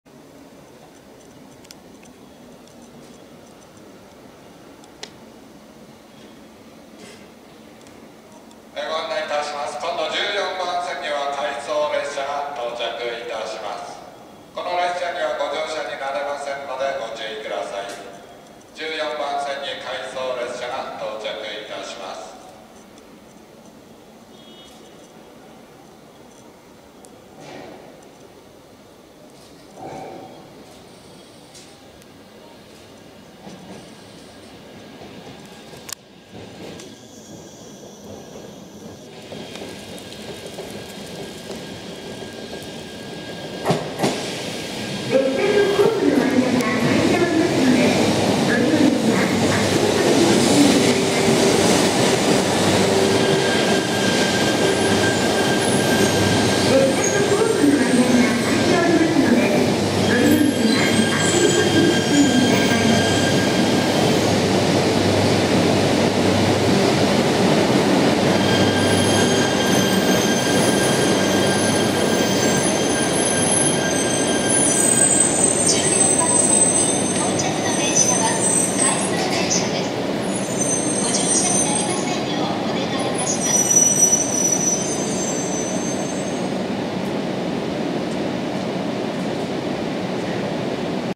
山形新幹線E3系とれいゆ 福島駅到着 E3-700 series "TOREIYU" Excursion Set
普段は入線しない福島駅東北新幹線ホームに入線するリゾート列車とれいゆ。
East Japan Railway Yamagata Shinkansen E3 series EMU train will arrive at Fukushima station.
東日本旅客鐵道山形新幹線E3型電聯車將到達福島車站。
동일본 여객철도 야마가타 신칸센 E3계 전동차는 후쿠시마 역에 도착합니다.
ကျည်ဆန်ရထားကျည်ဆန်ရထားဖူကူရှီးမားဘူတာရုံမှာရောက်လာပါလိမ့်မယ်။